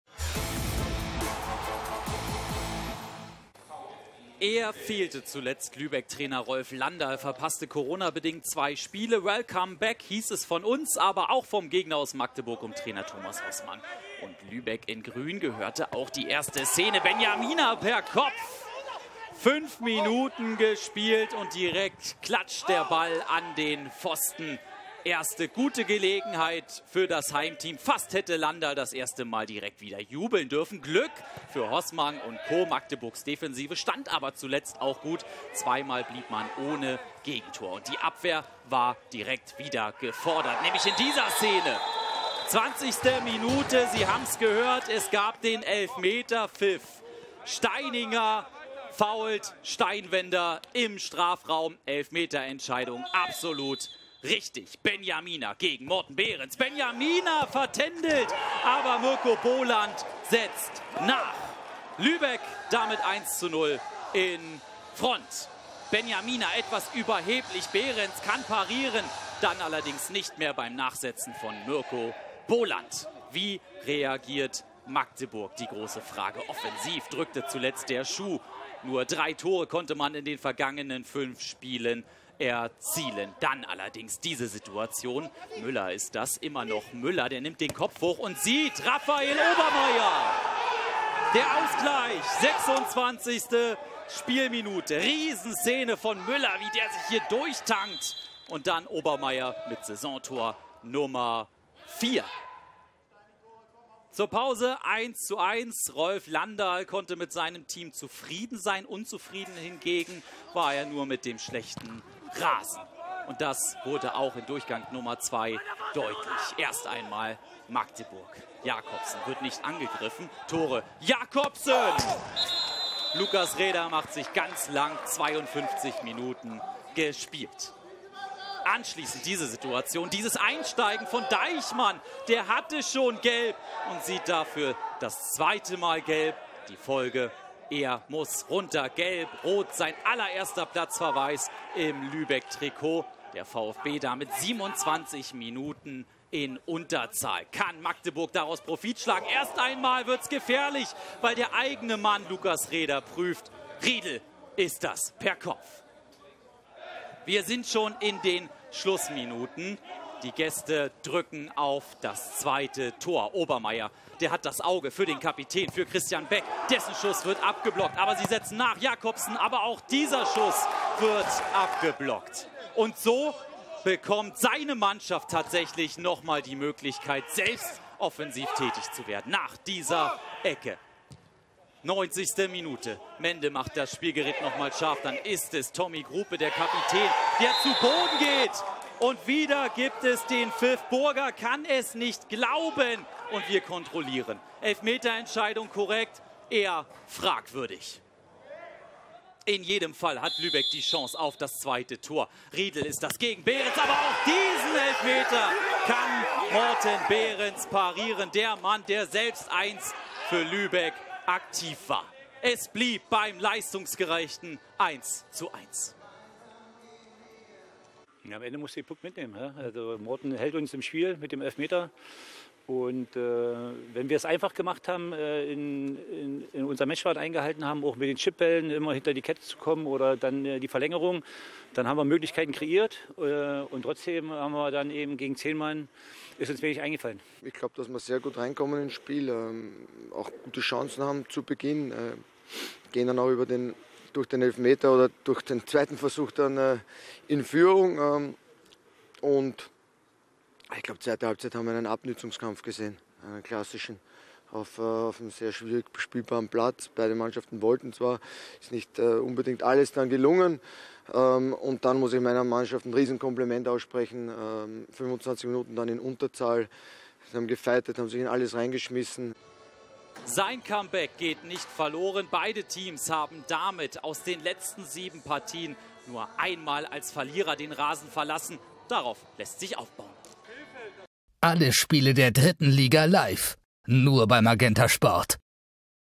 VfB Lübeck - 1. FC Magdeburg | 15. Spieltag, 2020/2021 | MAGENTA SPORT
Alle Spiele der 3. LIGA live bei MagentaSport:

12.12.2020, 15. Spieltag, 20/21. Lübeck und der 1. FC Magdeburg trennen sich 1:1. Lübeck ist damit seit nunmehr sechs Spielen ohne Sieg. Mirko Boland bringt die Gastgeber Mitte der ersten Hälfte in Führung, Raphael Obermair gleicht aber wenig später für Magdeburg aus.

Bleib' immer am Ball und verpasse keine Highlights mehr: MagentaSport MagentaSport auf YouTube MagentaSport auf Facebook MagentaSport auf Instagram